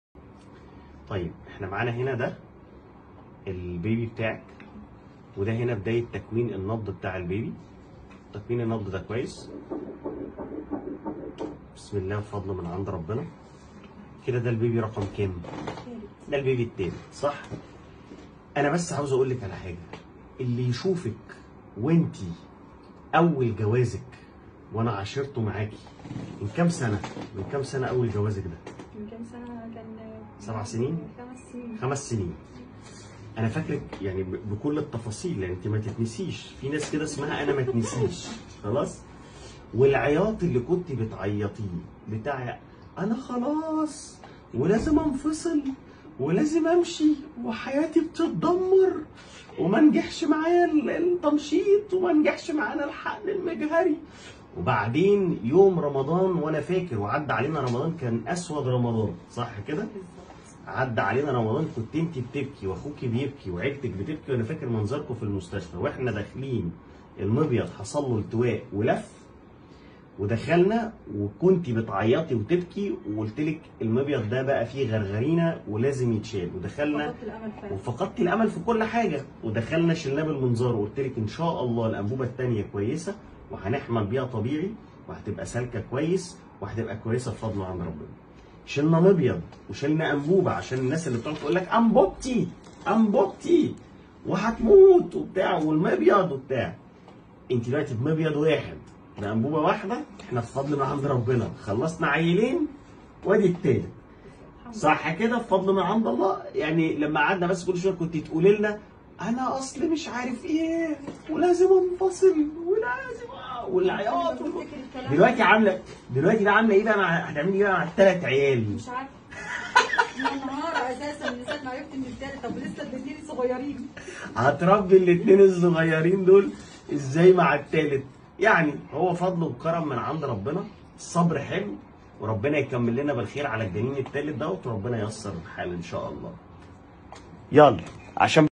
بعد استئصال مبيض وانبوب فالوب بالمنظار حملت ثلاث مرات
بعد المنظار …. العيل التالت بفضل الله بانبوبة واحدة و مبيض واحد .
والله نفسي تصوركم وانتوا فاقدين الامل وقالبين الدنيا كلها دراما واكتئاب وبعد ما ربنا بيكرمكوا.
او التواصل من خلال الارقام التالية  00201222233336 او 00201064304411